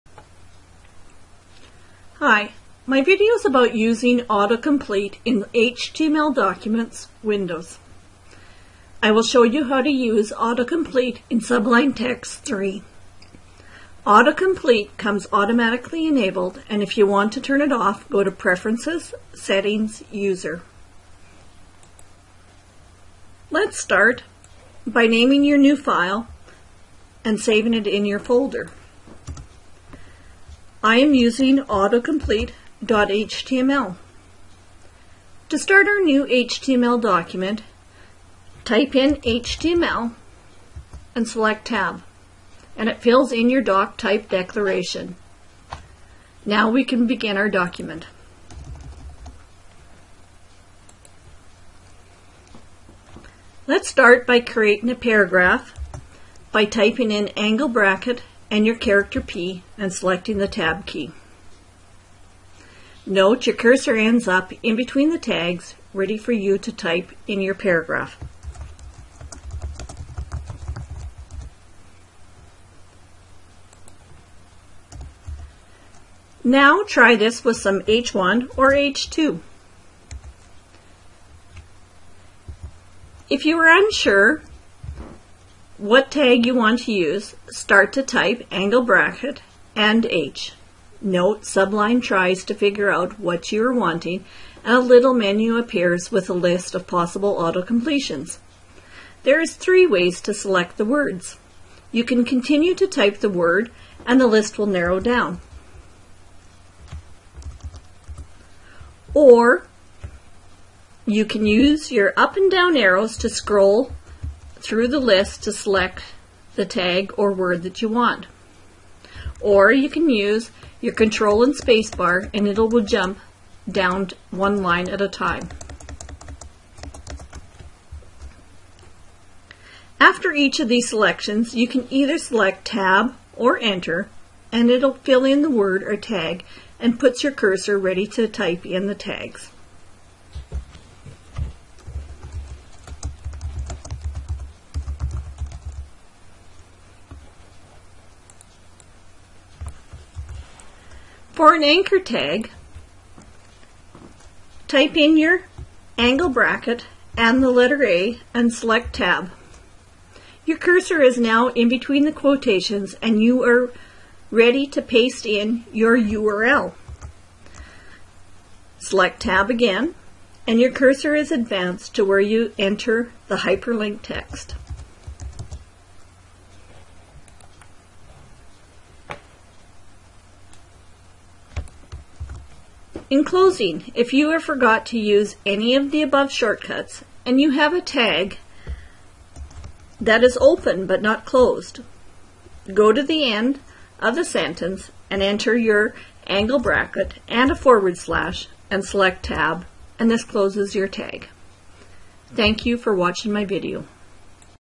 Sublime Text Auto Complete
This is a video on tips and tricks for auto complete for Sublime Text 3.